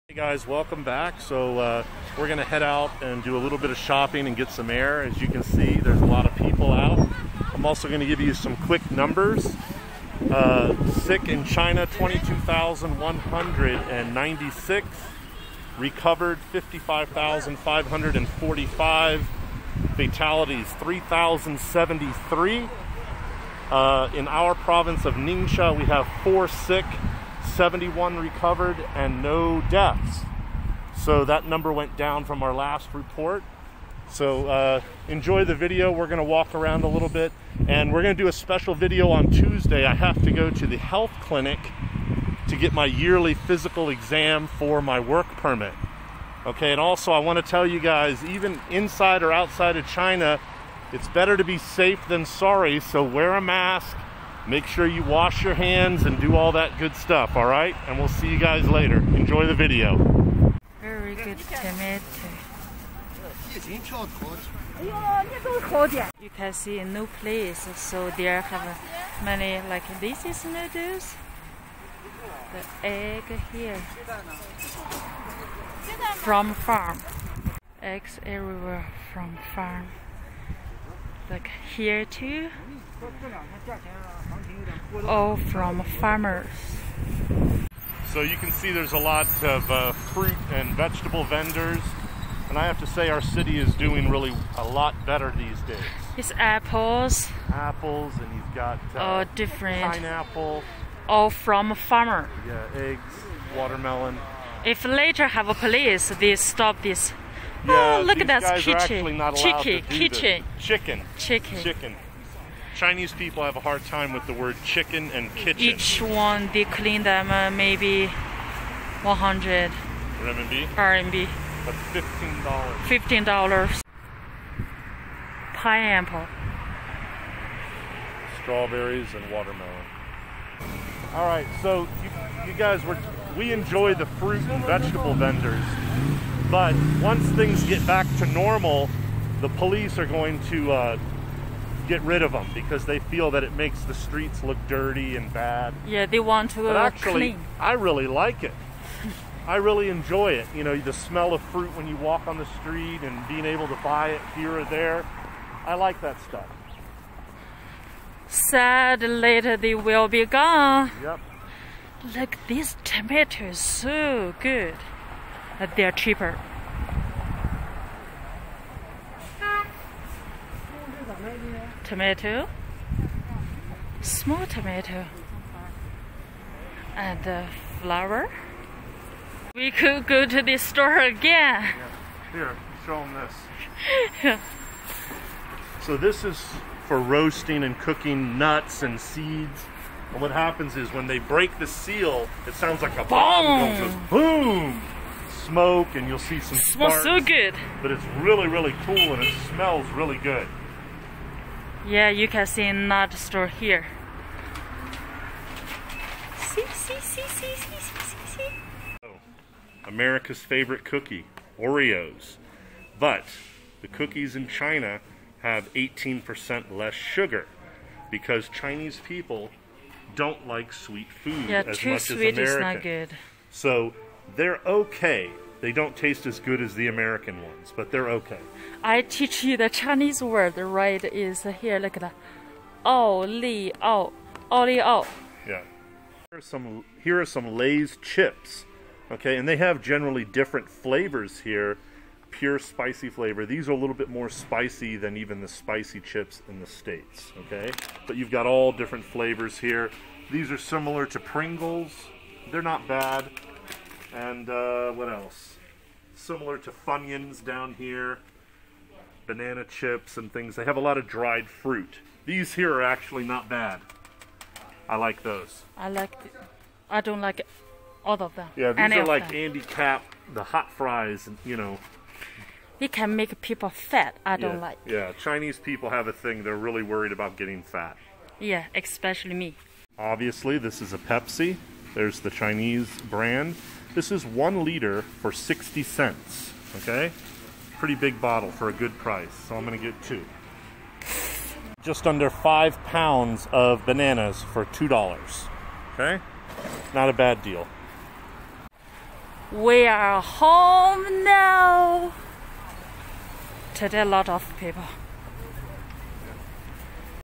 COVID-19 - Day 33 - Update from China
A quick update of the numbers for the COVID-19 virus out of China, and a quick walk to the grocery store.